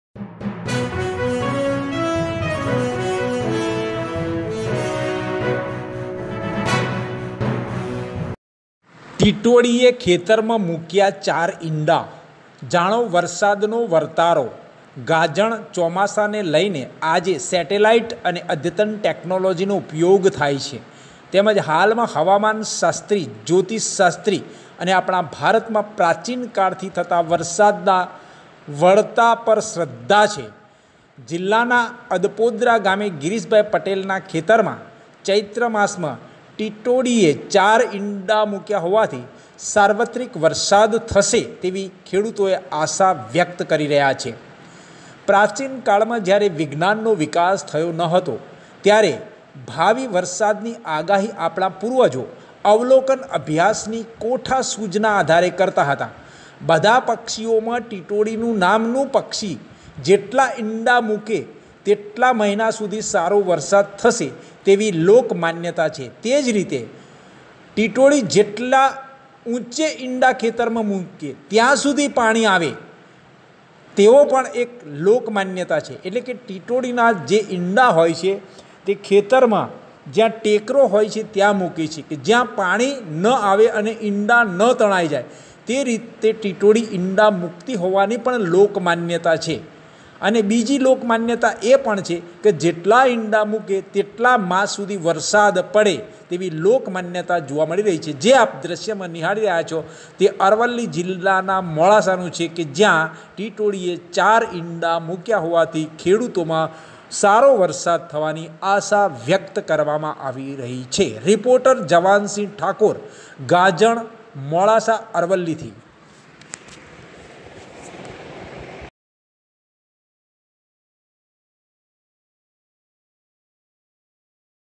ટીટોડીએ ખેતરમાં મૂક્યા ચાર ઈંડા, જાણો વરસાદનો વરતારો. ગાજણ.
𝗟𝗮𝘁𝗲𝘀𝘁 𝗡𝗲𝘄𝘀